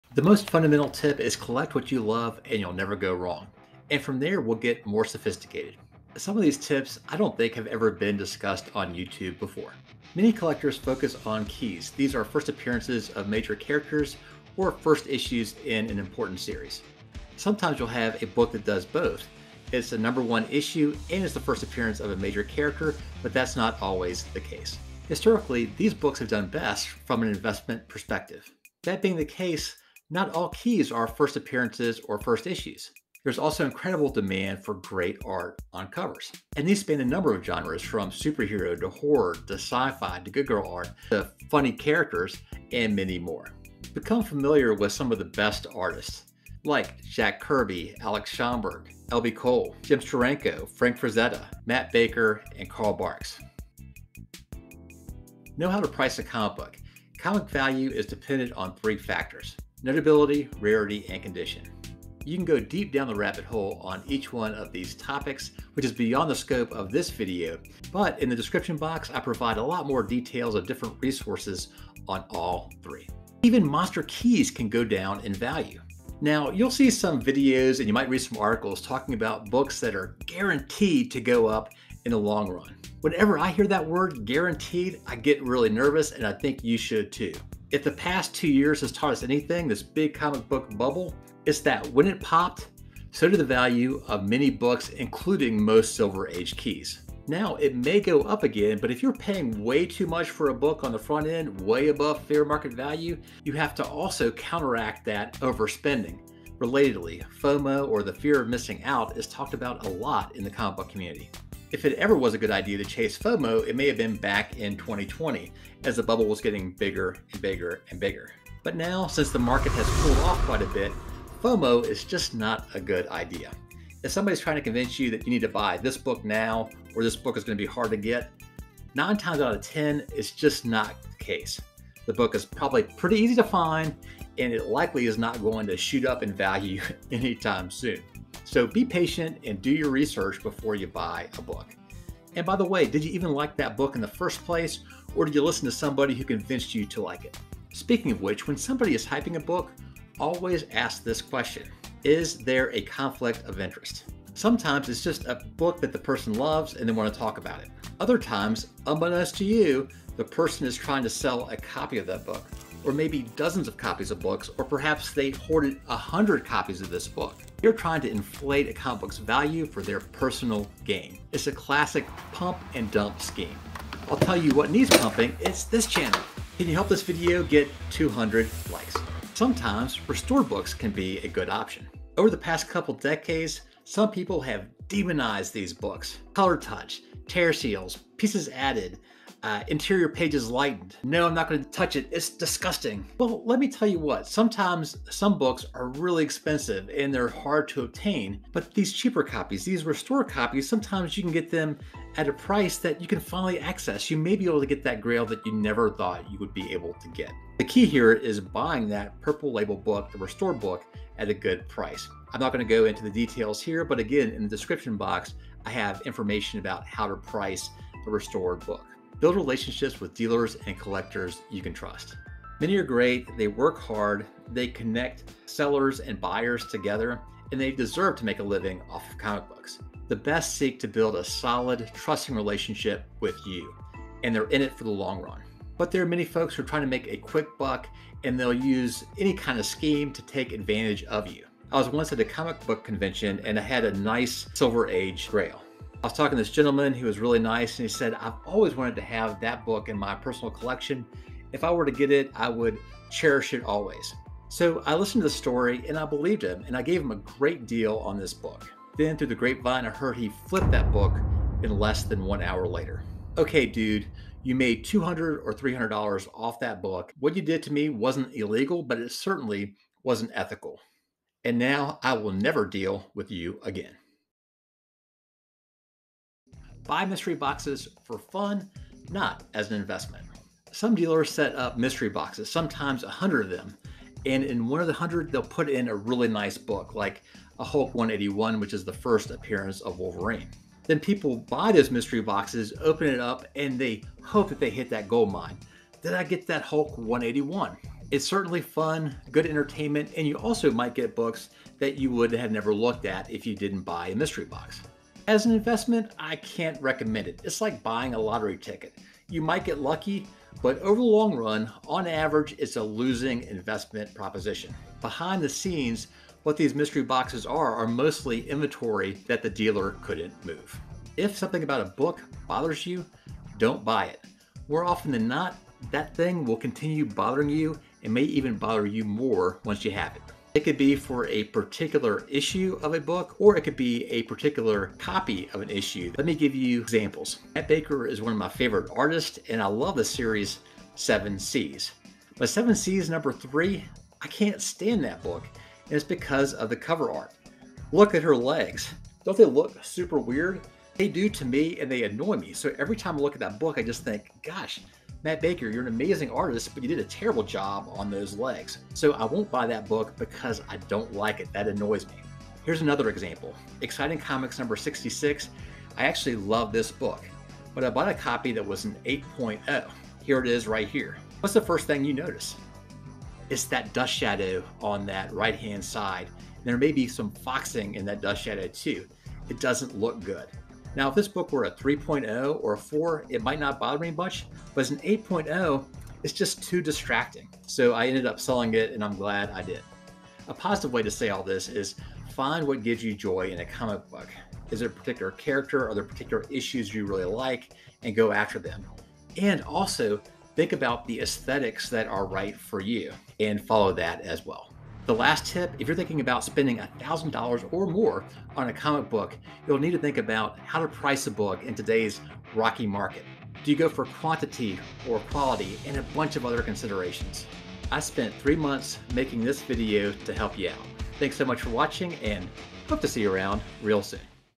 Collecting Comic Books: Advice from BASIC to ADVANCED
Are comic books guaranteed to go up? Should you buy mystery boxes as investments?  Are 1st appearances the only keys? Get advice on these questions and more in this video.

---RESOURCES---
**Most Valuable Comic Books**

**Best Cover Art**

  Best Artists.

**History of Comic Book Prices***

Acknowledgements

Images of Comic Books (and the $5,000 bill) are from Heritage Auctions (Ha.com).

GPAnalysis used for most images/videos of comic book pricing.